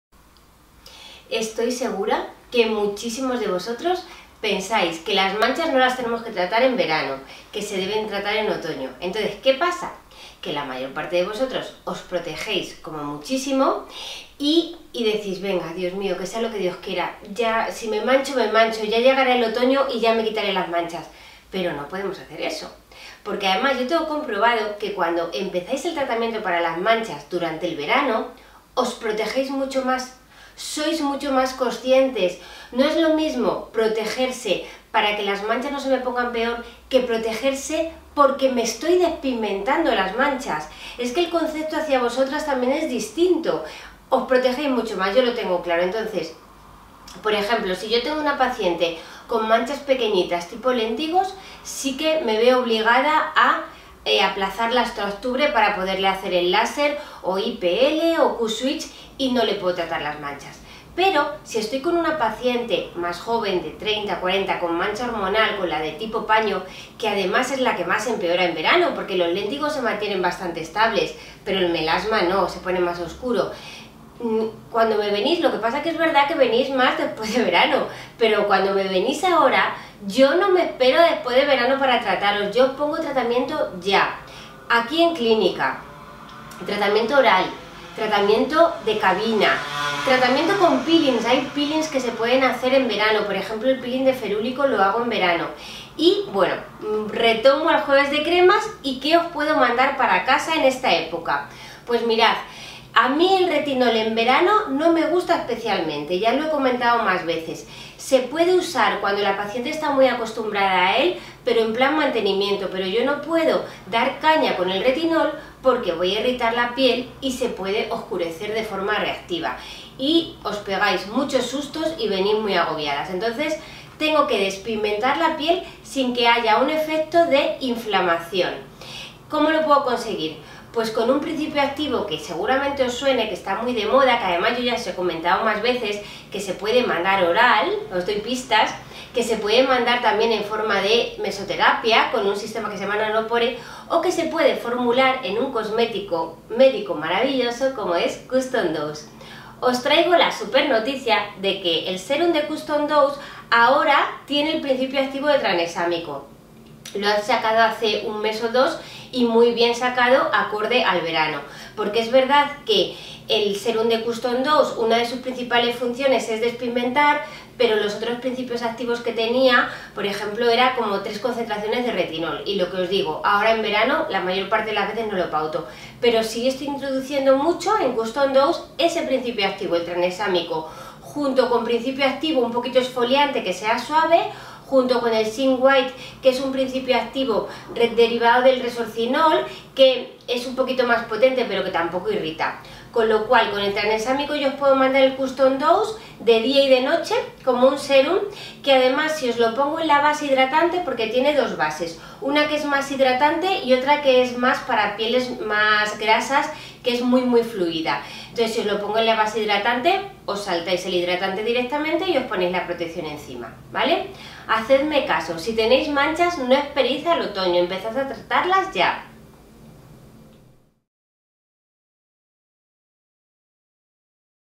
Tratamientos de Verano para Melasma - JuevesDeCremas | Dermaforyou by Dra Carmen Galera
Sigo pensando que muchos de vosotros pensáis que las manchas no se pueden tratar en verano, ¿Verdad?
Seguro que la mayoría de vosotros os protegéis (como mucho) y, bueno.. que sea lo que Dios quiera, si me mancho ya me trataré las manchas en otoño…

Pero tengo claro que, cuando empezáis los tratamientos antimanchas en verano, os protegéis mucho más!
Es verdad que si tenéis manchas pequeñas tipo léntigos, los tratamientos los empezamos en otoño porque este tipo de manchas se quitan a través de láser IPL o Q-Switched

Pero si se trata de manchas hormonales tipo paño que además es la que más empeora en verano, suelen ponerse más oscuras, las empiezo a tratar en el mismo verano (aunque soléis venir después de que pase la temporada).

En Clínica se comenzaría con tratamiento oral, tratamientos en cabina, tratamiento con peelings! Hay Peelings que se pueden hacer en verano!! Y bueno, ya que estamos en JuevesDeCremas, os quiero hablar de una de mis favoritas!!
Ya sabéis que no soy muy fan de usar retinol en verano, me parece bien que se utilice cuando la paciente está acostumbrada a él y se utiliza por mantenimiento, pero no para comenzar su tratamiento en verano porque la piel se va a irritar y es fácil que se oscurezca de forma reactiva.

Entonces, lo que tengo que hacer es despigmentar la piel sin que haya un efecto de inflamación.. ¿Cómo lo consigo?
➡️ Echa un vistazo al video entero!

PRODUCTO:
Custom D.O.S.E de Skinceuticals